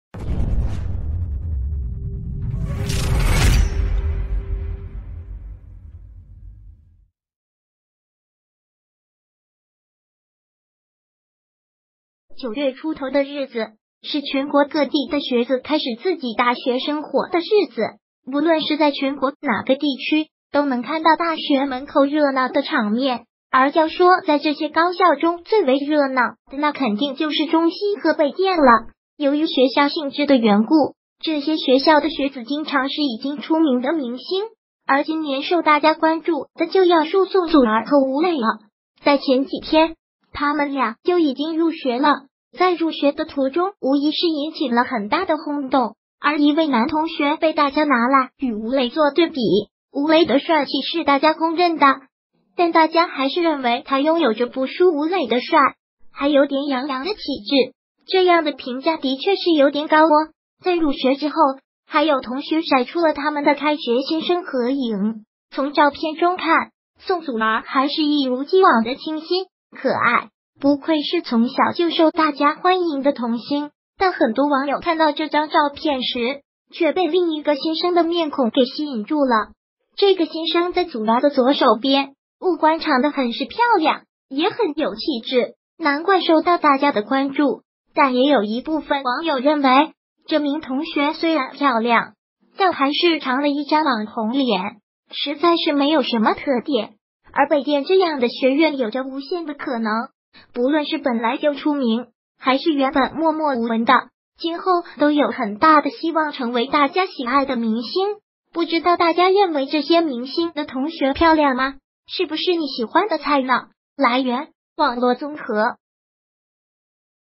北电新生合影 宋祖儿和吴磊很美 但他们的同学更引人瞩目
九月出头的日子，是全国各地的学子开始自己大学生活的日子，不论是在全国哪个地区，都能看到大学门口热闹的场面。而要说在这些高校中最为热闹的那肯定就是中戏和北电了，由于学校性质的缘故，这些学校的学子经常是已经出名的明星。而今年受大家关注的就要数宋祖儿和吴磊了，在前几天，他们俩就已经入学了，在入学的途中无疑是引起了很大的轰动。而一位男同学被大家拿来与吴磊作对比，吴磊的帅气是大家公认的，但大家还是认为他拥有着不输吴磊的帅，还有点杨洋的气质，这样的评价的确是有点高哦！在入学之后，还有同学晒出了他们的开学新生合影，从照片中看，宋祖儿还是一如既往的清新、可爱，不愧是从小就受大家欢迎的童星。但很多网友看到这张照片时，却被另一个新生的面孔给吸引住了，这个新生在祖儿的左手边，五官长的很是漂亮，也很有气质，难怪受到大家的关注，但也有一部分网友认为，这名同学虽然漂亮，但还是长了一张网红脸，实在是没有什么特点。而北电这样的学院有着无限的可能，不论是本来就出名，还是原本默默无闻的，今后都有很大的希望成为大家喜爱的明星。不知道大家认为